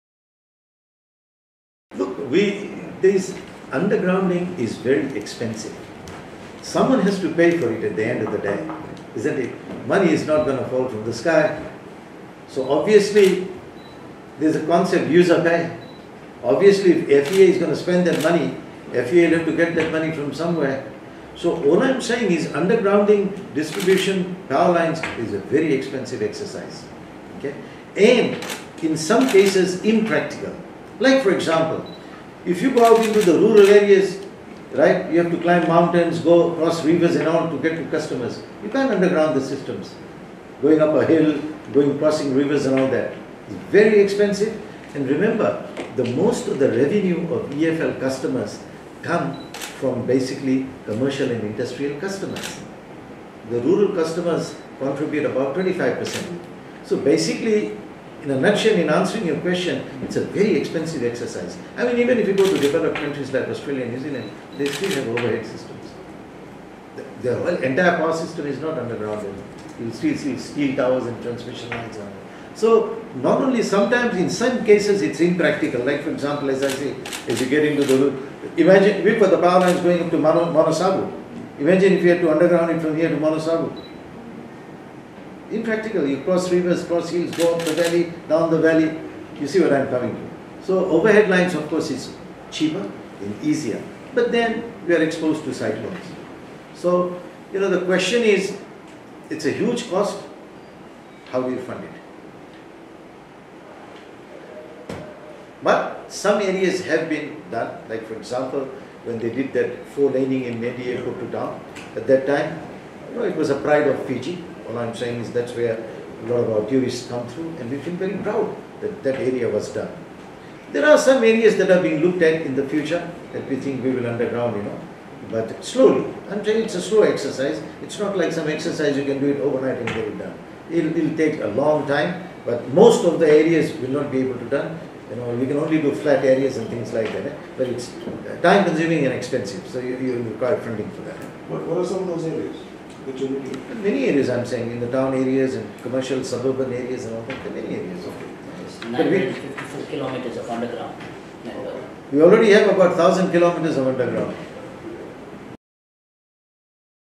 Undergrounding cables is expensive and someone will have to pay for it – Patel
Energy Fiji Limited says undergrounding which is the replacement of overhead power cables with underground cables is very expensive and in some cases can be impractical.

When asked if EFL has a long term plan to do undergrounding for more areas considering Fiji’s vulnerability to natural disasters, CEO Hasmukh Patel says at the end of the day someone has to be pay for it.

He says money is not going to fall from the sky.